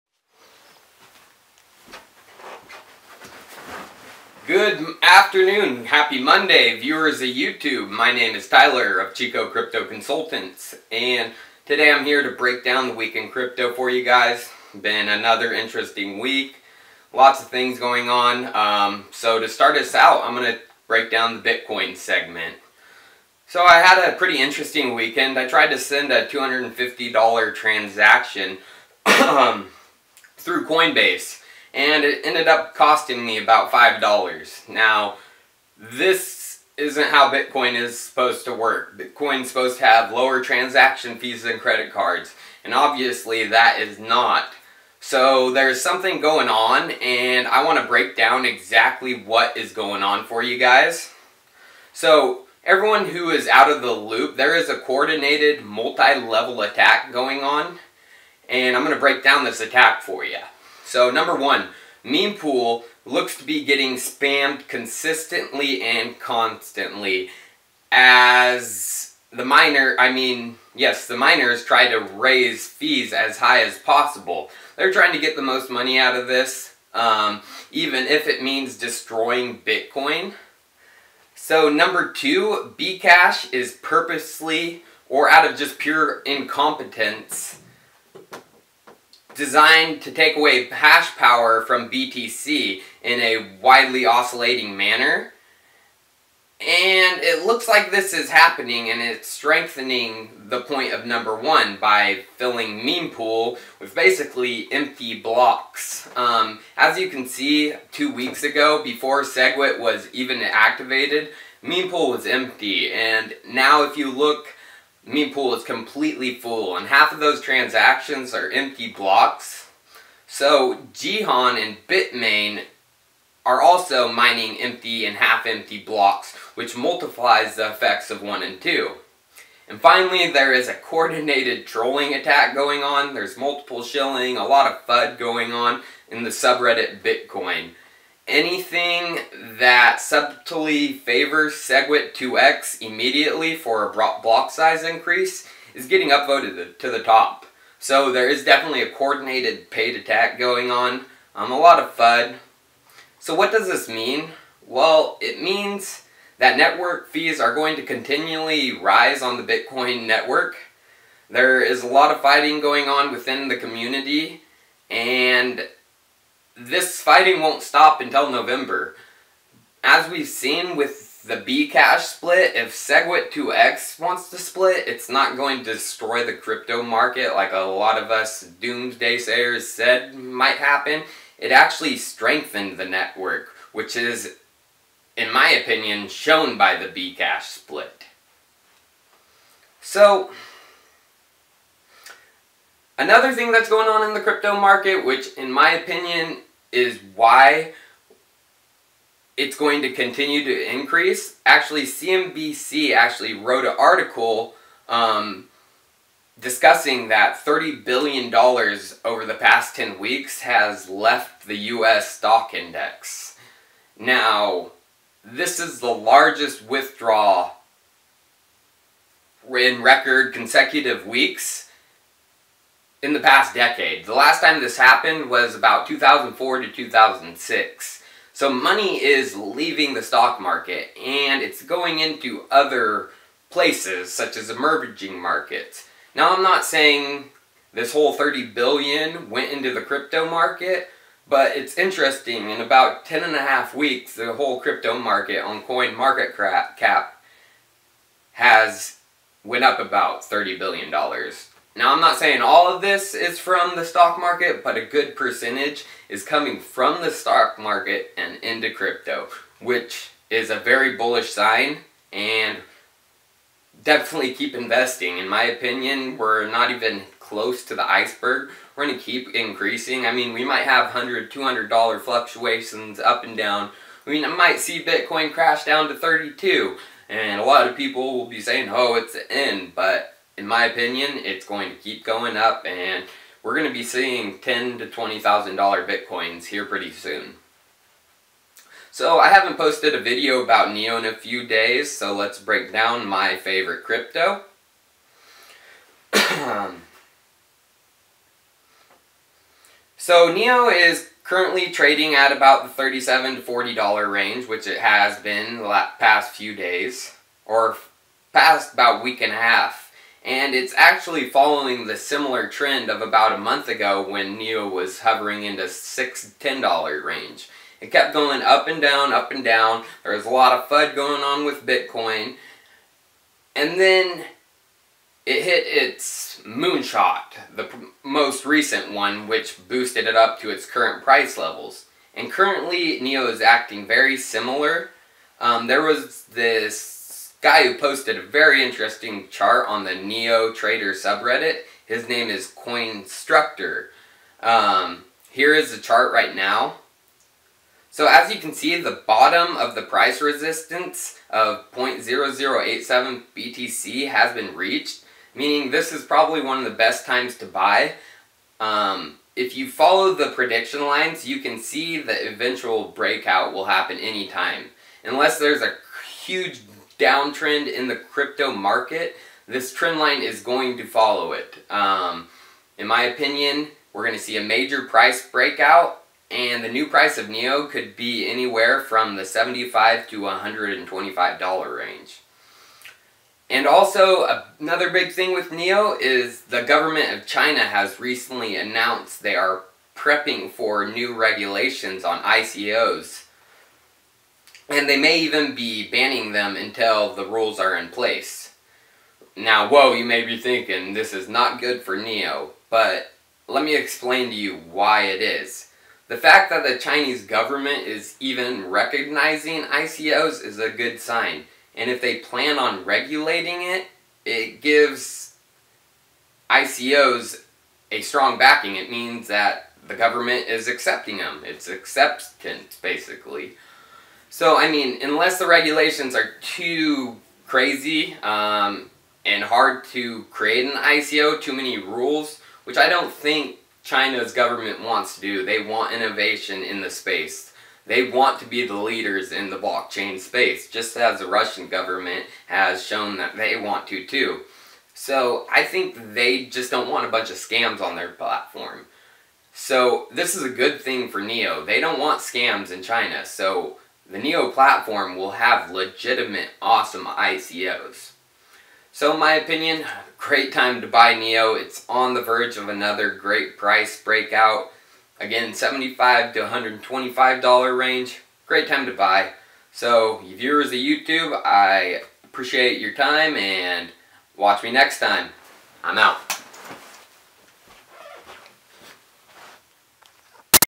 Neo Price Movements-$125 NEO soon? Crypto Market is not in a Bubble!!
Discussion of the crypto market, and why money is moving from the U.S Stock Index and into crypto, and an analysis of the current NEO price and why I think it will break out soon!